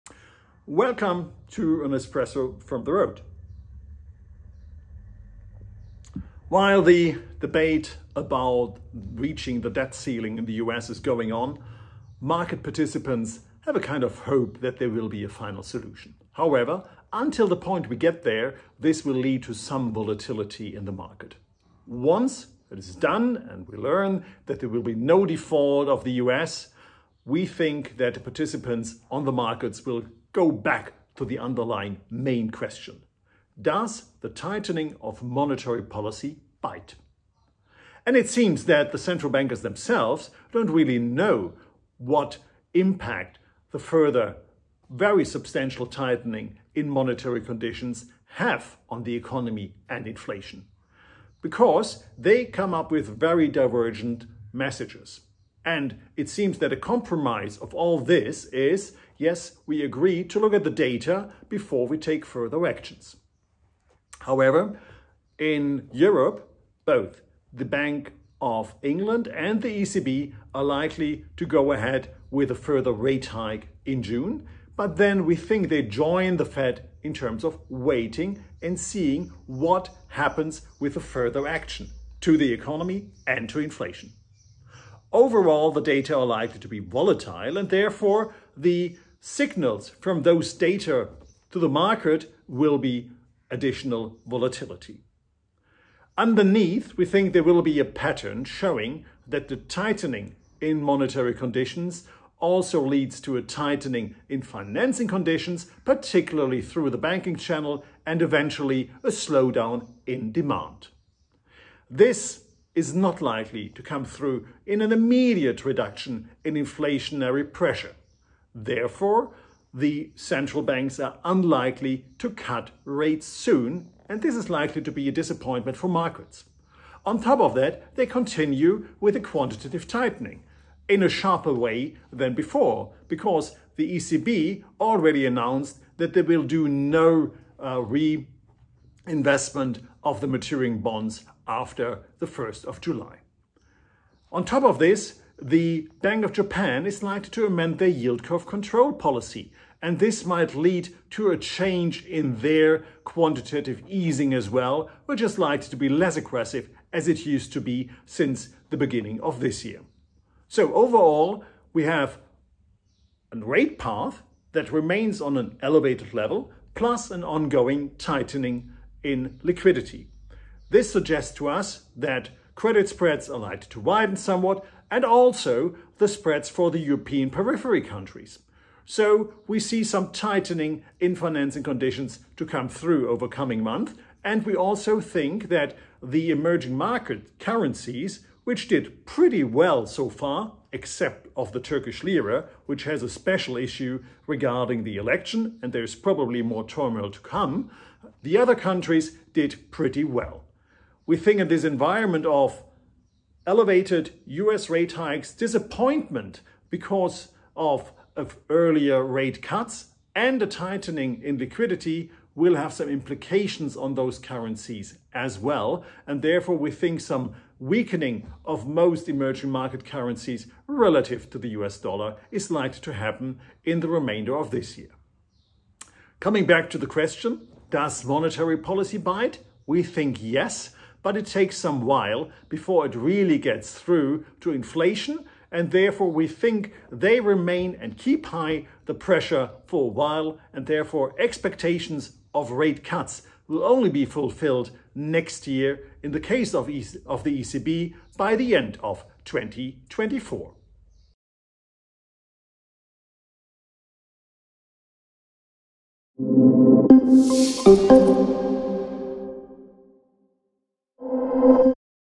Monetary tightening is beginning to bite
BayernLB-chief economist Jürgen Michels argues the key rate hikes implemented by central banks are increasingly biting and dampening aggregate demand. Where key policy rates are now plateauing out, central-bank liquidity is continuing to be tightened.